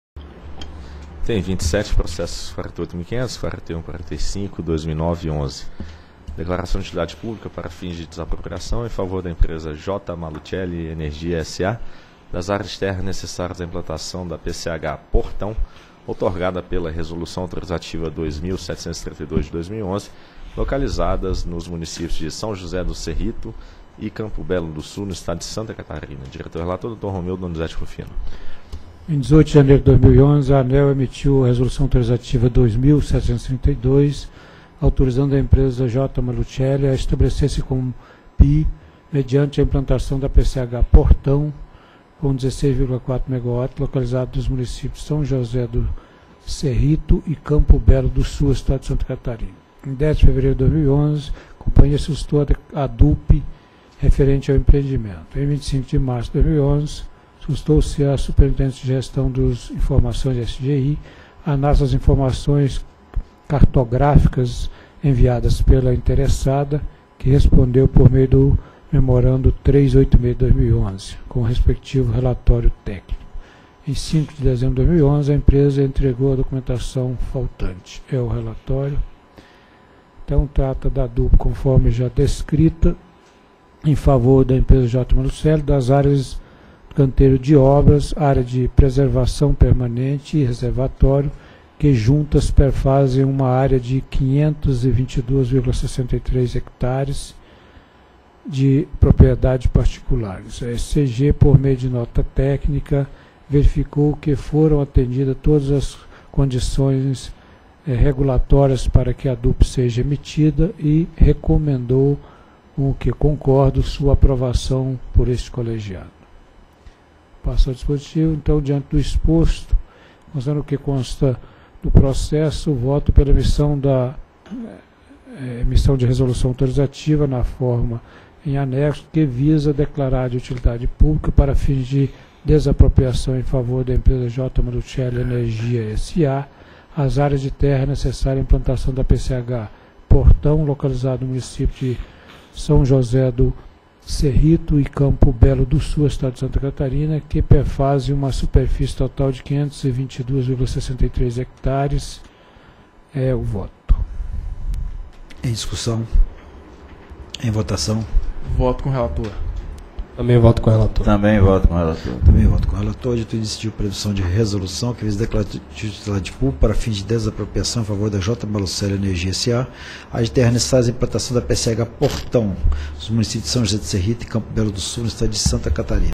Item 27 - 1ª Reunião Pública Ordinária ANEEL 17/01/2012
Item 27. Processo nº: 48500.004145/2009-11. Assunto: Declaração de utilidade pública, para fins de desapropriação, em favor da empresa J. Malucelli Energia S.A., das áreas de terra necessárias à implantação da Pequena Central Hidrelétrica -- PCH Portão, outorgada pela Resolução Autorizativa nº 2.732/2011, localizadas nos municípios de São José do Cerrito e Campo Belo do Sul, no estado de Santa Catarina. Área Responsável: Superintendência de Concessões e Autorizações de Geração -- SCG.